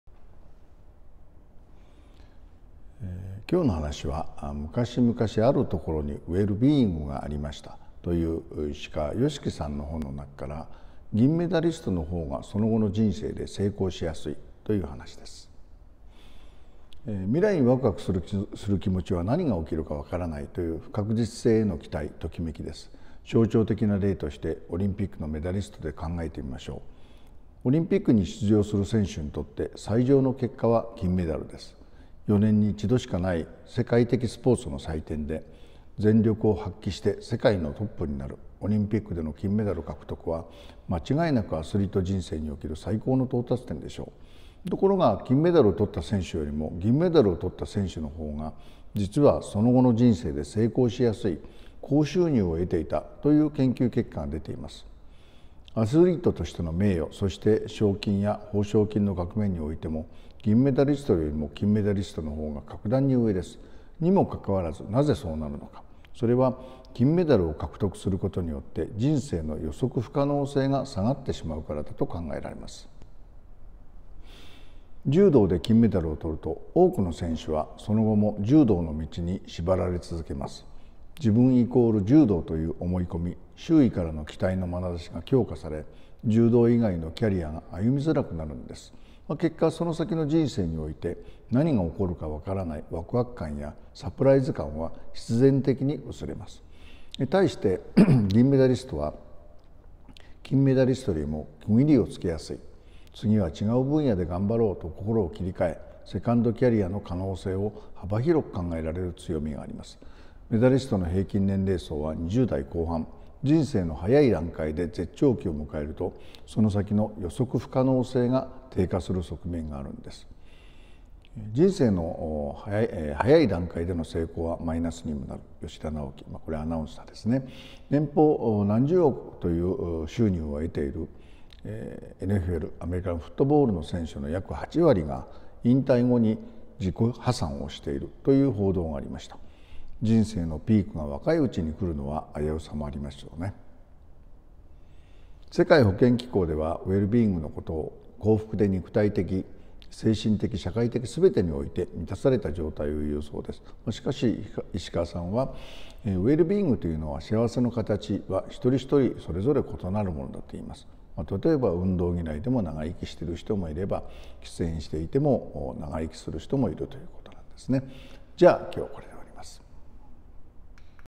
【銀メダリストのほうがその後の人生で成功しやすい】（石川善樹・よしき）
1.未来にワクワクする気持ちは、「何が起きるかわからない」という不確実性への期待、ときめきです。
＊象徴的な例として、オリンピックのメダリストで考えてみましょう。 オリンピックに出場する選手にとって、最上の結果は金メダルです。＊4年に1度しかない世界的スポーツの祭典で、全力を発揮して世界のトップになる。オリンピックでの金メダル獲得は、間違いなくアスリート人生における最高の到達点でしょう。
2.ところが、金メダルを取った選手よりも、銀メダルを取った選手のほうが実はその後の人生で成功しやすい、高収入を得ていたという研究結果が出ています。
＊アスリートとしての名誉、そして賞金や報奨金の額面においても銀メダリストよりも金メダリストのほうが格段に上です。にもかかわらず、なぜそうなるのか? ＊それは金メダルを獲得することによって、人生の「予測不可能性」が下がってしまうからだと考えられます。

3.柔道で金メダルを取ると、多くの選手はその後も柔道の道に縛られ続けます。
＊自分 =柔道という思い込み、周囲からの期待の眼差しが強化され、柔道以外のキャリアが歩みづらくなるのです。
＊結果、その先の人生において何が起きるかわからないワクワク感やサプライズ感は必然的に薄れます。

4.対して、銀メダリストは金メダリストより区切りをつけやすい。
＊「次は違う分野で頑張ろう」と心を切り替え、セカンドキャリアの可能性を幅広く考えられる強みがあります。
＊メダリストの平均年齢層は20代後半。人生の早い段階で絶頂期を迎えると、 その先の予測不可能性が低下する側面があるのです。

5.《人生の早い段階での成功はマイナスにもなる》（吉田尚記）
＊年棒何十億円という収入を若いうちに得るNFL(アメリカンフットボール)選手の約8割が引退後に自己破産している、という報道がありました。
＊人生のピークが若いうちに来るのは危うさもありますよね。

◆世界保健機関（WHO）では、ウェルビーイングのことを、幸福で肉体的、精神的、社会的すべてにおいて、満たされた状態をいうそうです。
＊しかし、石川善樹氏は、ウェルビーイングという「幸せのかたち」は、一人ひとりでそれぞれ異なるものだと言います。
＊たとえば、運動嫌いでも長生きしている人もいれば、喫煙していても長生きする人もいます。

◆また、「飽（あ）きる」というのは日本ではネガティブに捉えられがちですが、実は「飽きる」のは一種の才能だと石川氏は言います。
＊なぜなら、飽きやすい人のほうが、圧倒的に「新しいこと」を始めやすいからです。
＊人間の脳は「予測不可能」、つまり何が起こるかわからない未来を好む傾向があるので、「新しいこと」にワクワクするのですね。

◆金メダリストより、銀メダリストのほうが、人生の「予測不可能性」が高まる…
＊何が起こるかわからない予測不可能な未来を、ワクワク・ドキドキしながら楽しみたいと思います。